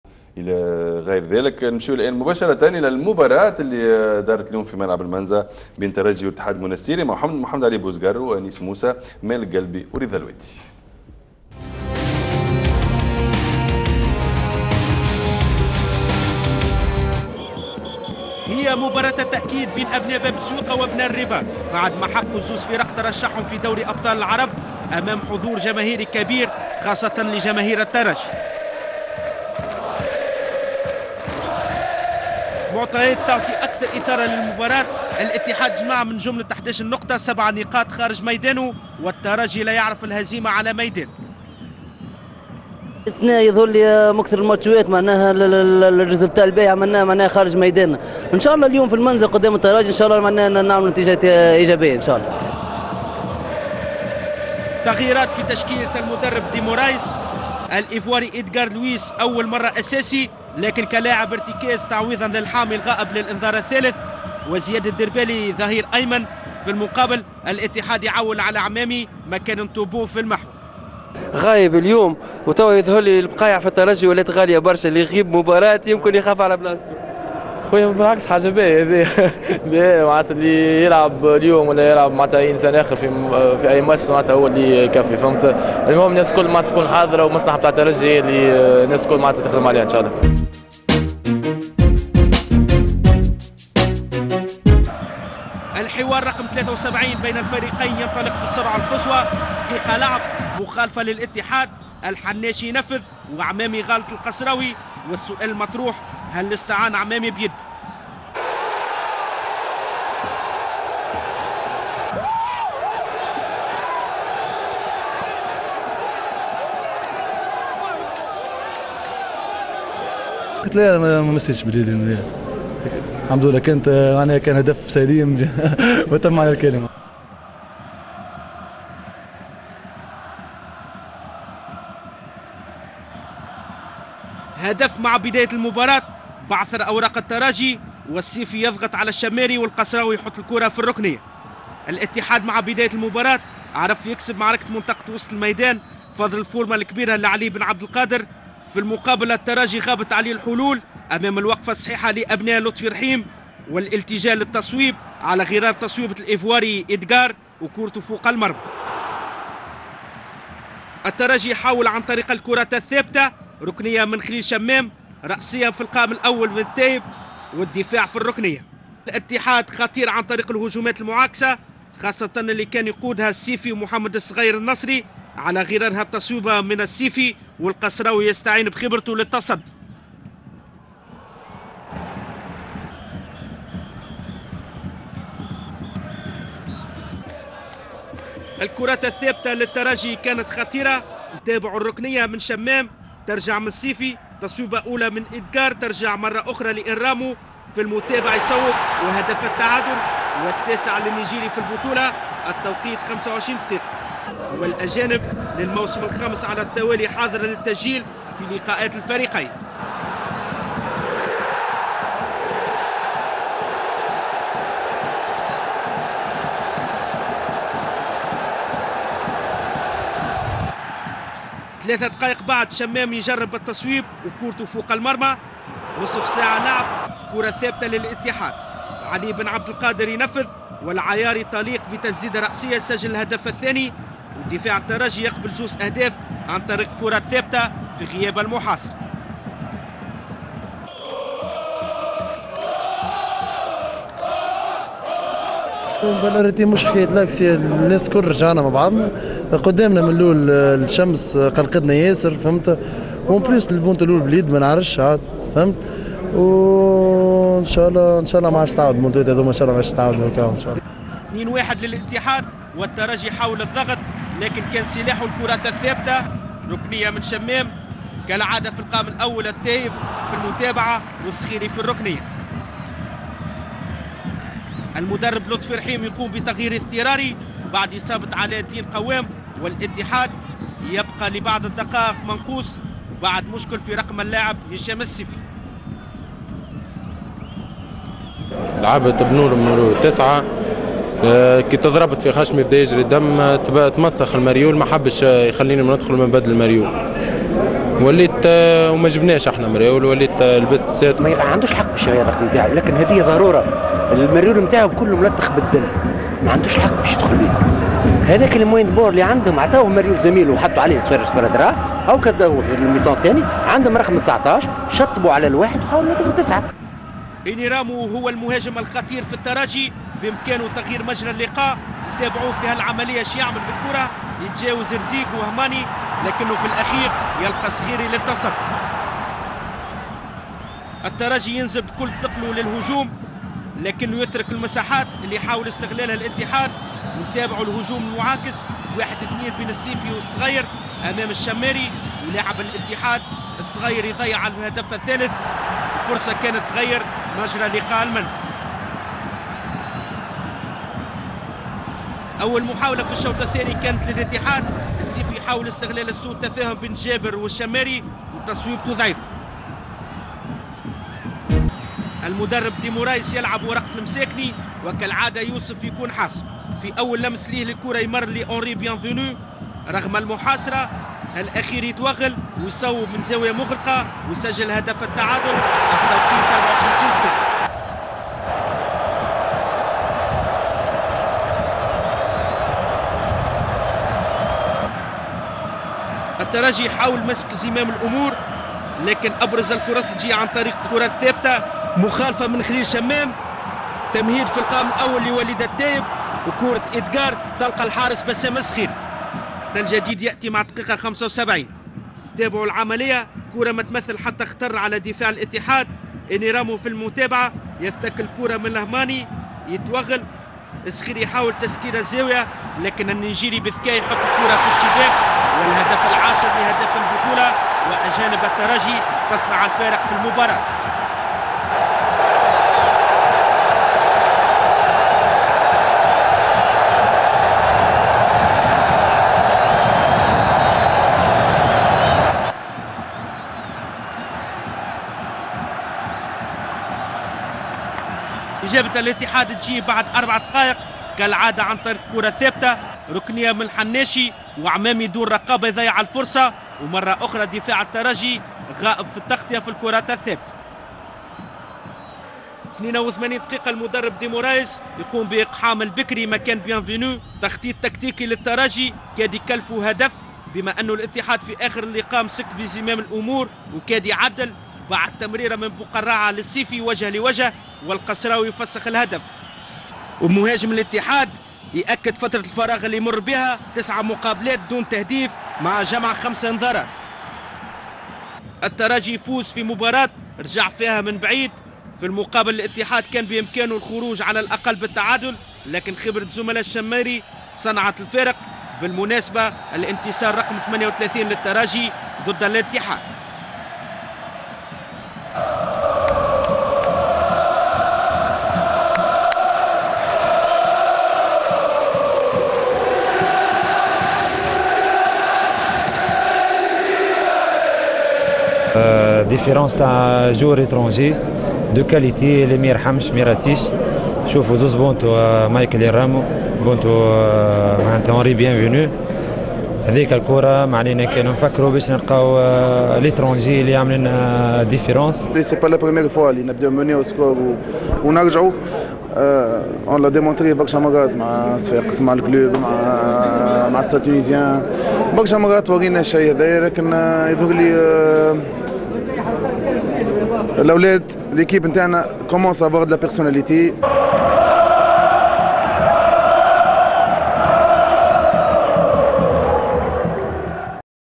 ملخص روعة وأهداف مباراة الترجي والاتحاد المنستيري 3 - 2 يوم 30-11-2008
الأحد الرياضي : ملخص روعة  وأهداف مباراة الترجي والاتحاد المنستيري 3 - 2 يوم 30/11-2008  بملعب المنزه الجولة العاشرة
ريمونتدا رائعة من صنع مايكل اينرامو و هنري بيانفينو و إبداع المساكني
  Dimanche Sport : Résumé - Espérance Sportive de Tunis vs Union Sportive Monastirienne 3 -2 Remontada 10 ème Journée

Stade Olympique d’El Menzah: EST – USMO (3-2) mi-temps (2-1). Buts : Maher Hannachi (2’) et Aymen Ayari (30’) pour l’USMO. Michael Enéramo (26’ et 75’) et Henri Ntsama Bienvenu (54’) pour l’EST. Arbitrage de M. Chamseddine Lamti secondé par Mondher Latrech et Tarak Jelassi. 4ème arbitre : Ghazi Ben Ghézaiel. Commissaire de match : Ali Thabti.
EST : Hamdi Kasraoui (cap), Samah Derbali, Larbi Jabeur, Seifeddine Chammari, Edgard Loué Gnoleba, Khalil Chammam, Zinelabedine Souissi, Walid Tayeb, Oussama Darragi (Youssef Msakni 53’), Michael Enéramo, Henri Ntsama Bienvenu (Wissem Bekri 82’). Entraîneur : José Manuel Des Moraïs.
USMO : Bassem Skhiri, Aymen Ayari, Haythem Rzig (Abdelmajid Ben Belgacem 89’), Alaâeddine Kawam (Nizar Boukaraâ 36’), Chaker Hammali (Fradj Sassi 57’), Mohamed Sghaier Nasri, Mothoana Bokang, Ali Ben Abdelkader (cap), Maher Hannachi, Hichem Sifi, Mohamed Amine Ammami. Entraîneur : Lotfi Rhim